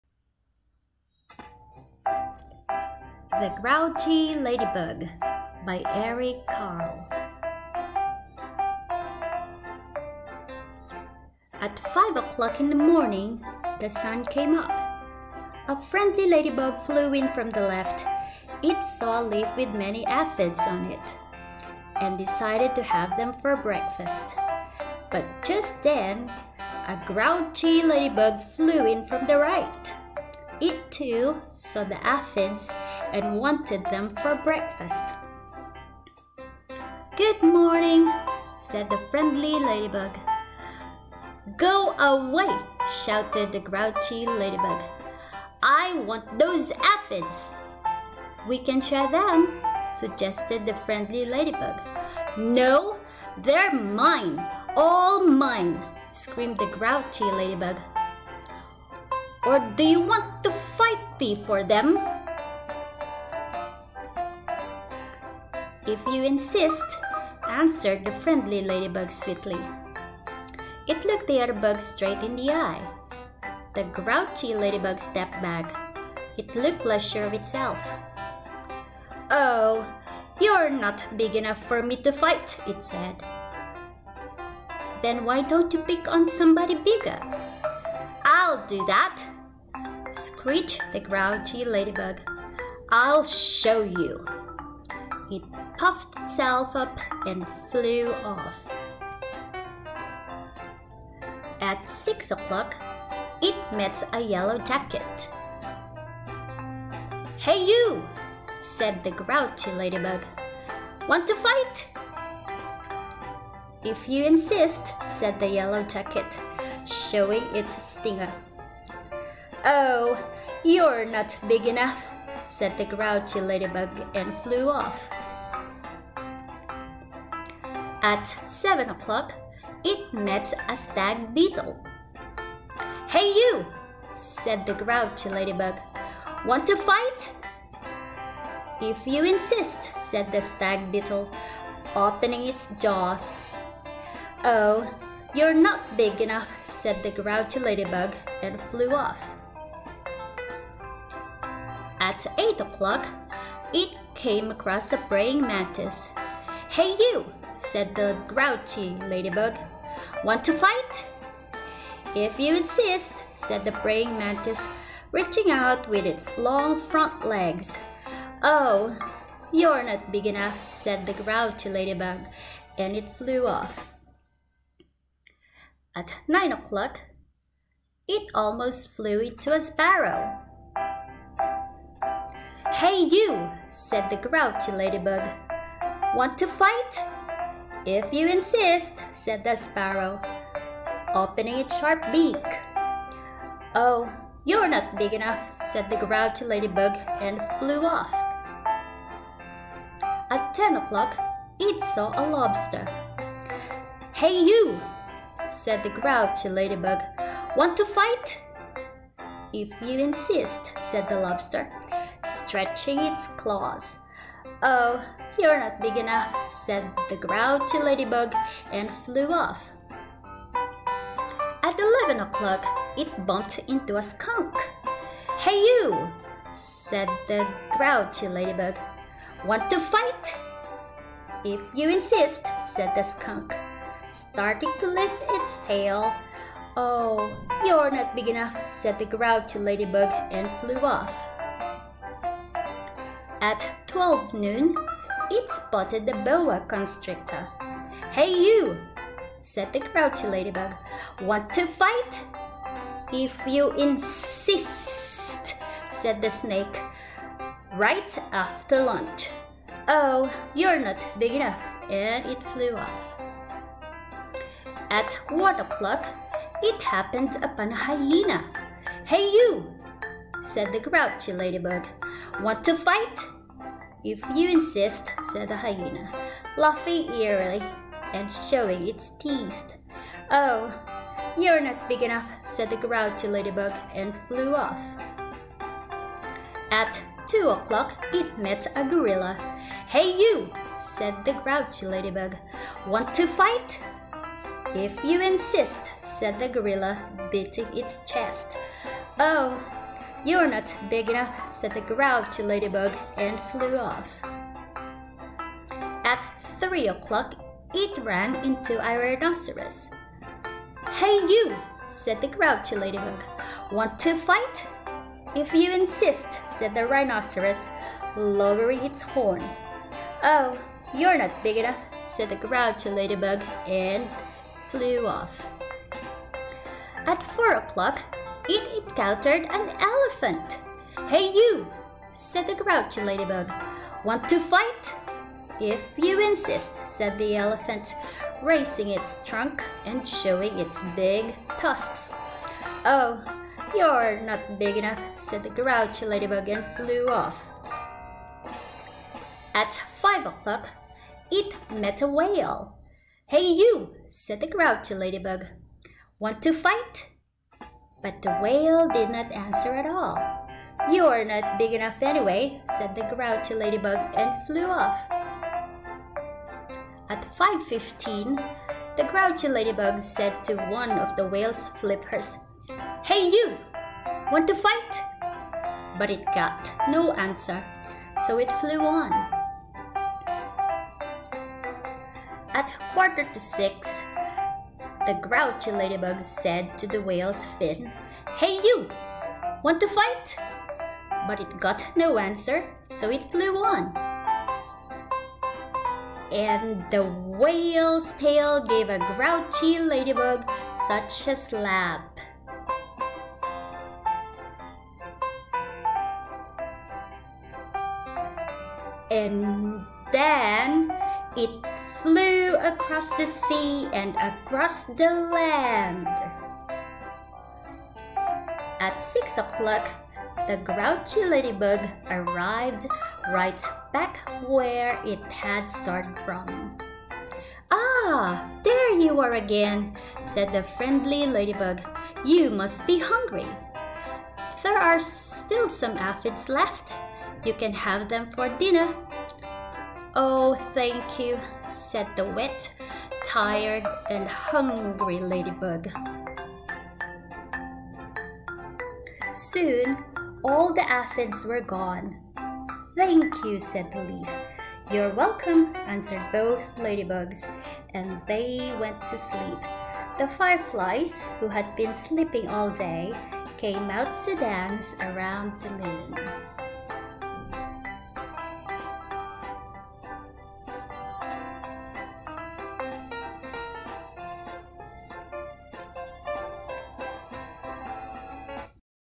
Animated The Grouchy Ladybug with visible analog clock
A unique Read Aloud story of The Grouchy Ladybug written by Eric Carle. What's make it unique? Watch till the end to find it out. I hope you enjoyed watching it.